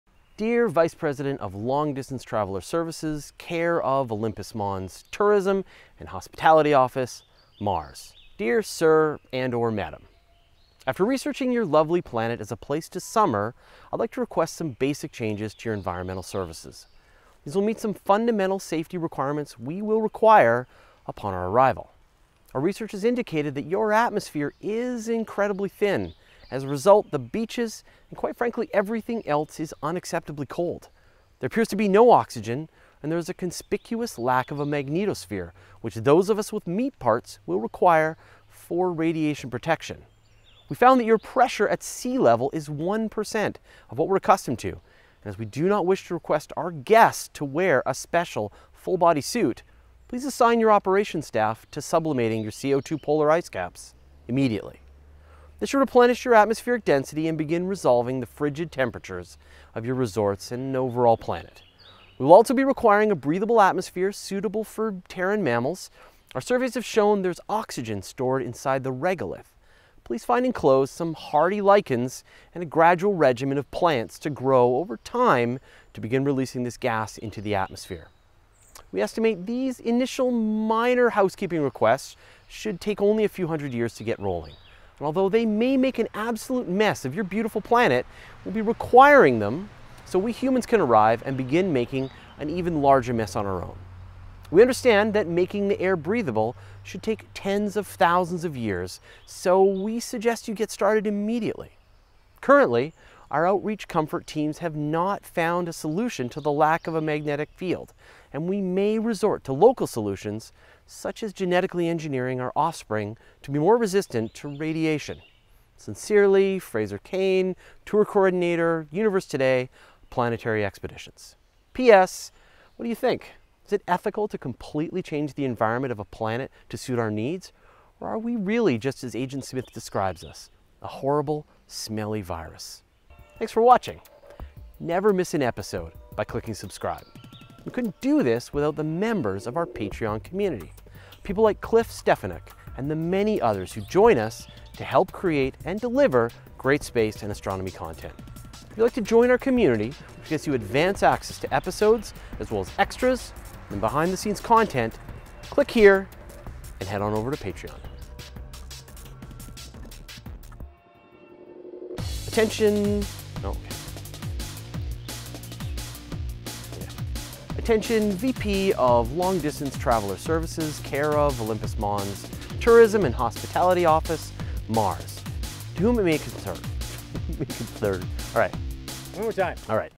How Do We Terraform Mars?
If we really want to live on Mars, we're going to need to do a complete renovation. We'll need to thicken the atmosphere, warm the planet, and get the liquid water flowing. What'll it take?